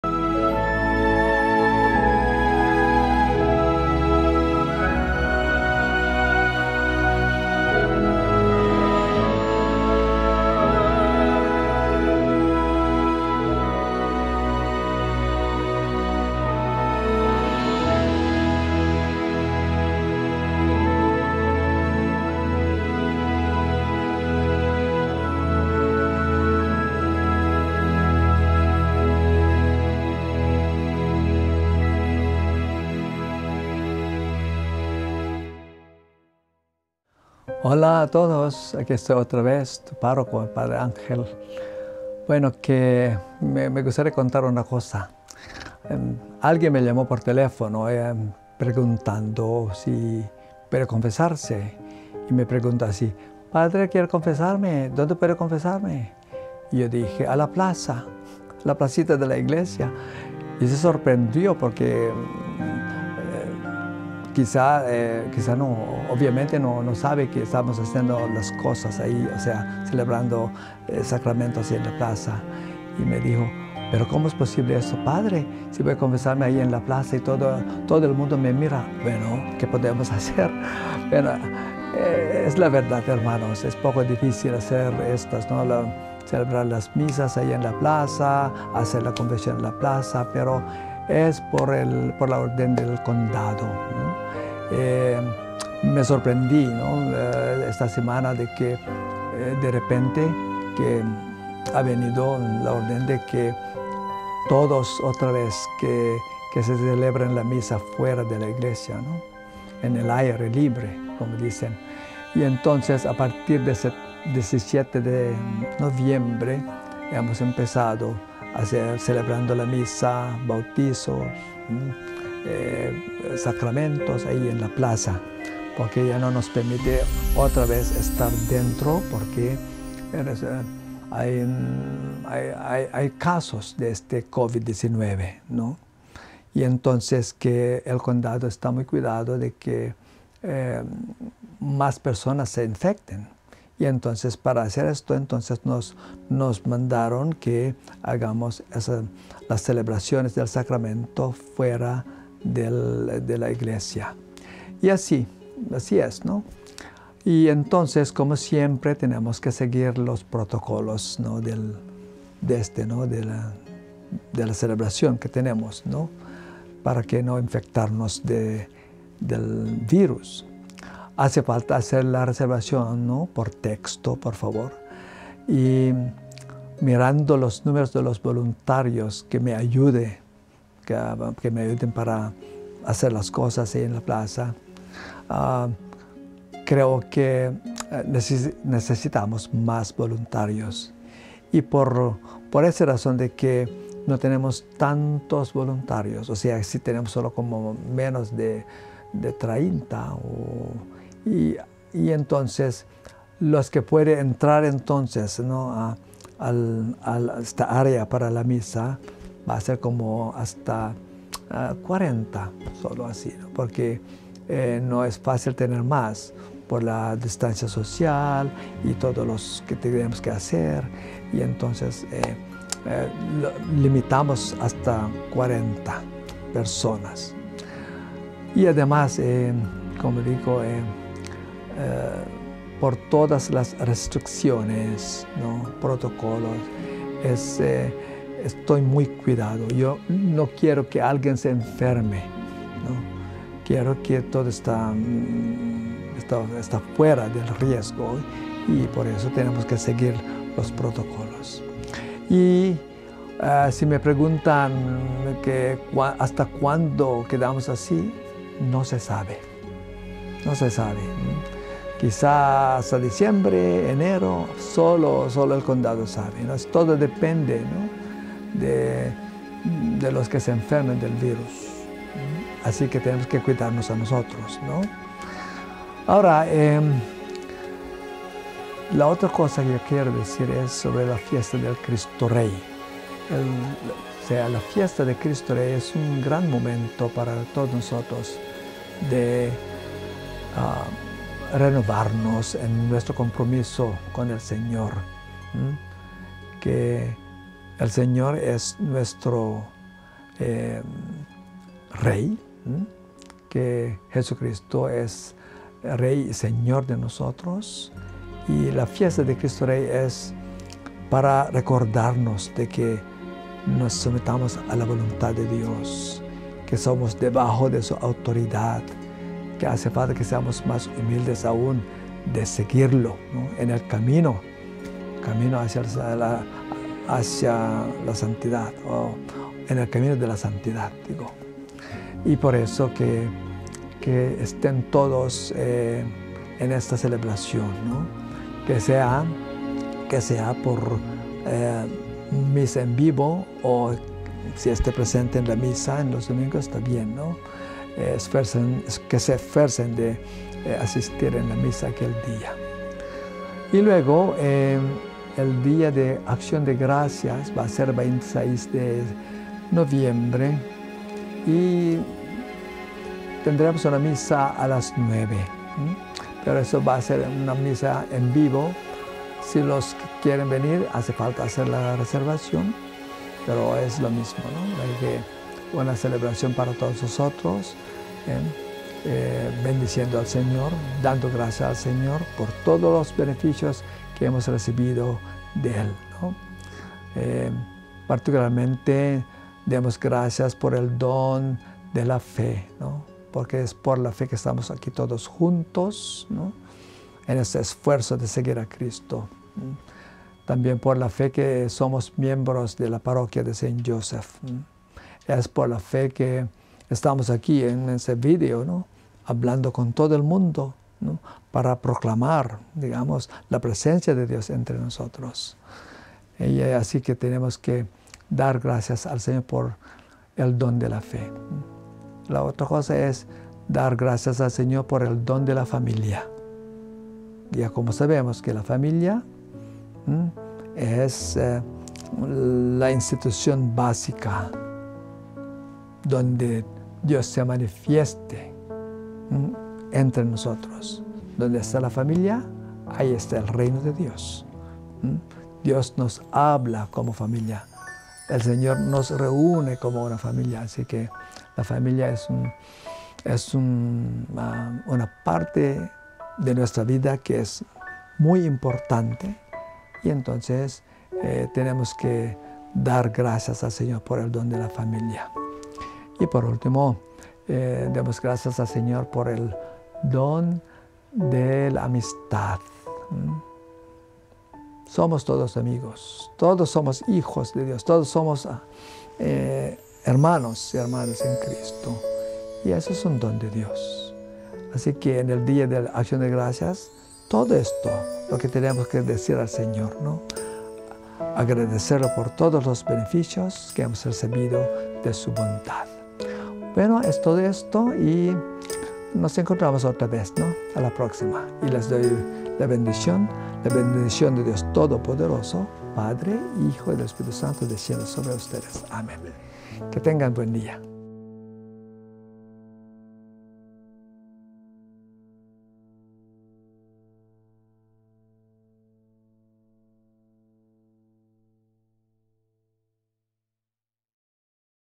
2020-11-22, Mensaje del Parroco (Español)
Mensaje de nuestro parroco, con avisos sobre los protocolos del Condado de Santa Clara debido a los casos actuales de COVID-19, y reflexiones del Día de Acción de Gracias y la Solemnidad de Nuestro Señor Jesucristo, Rey del Universo.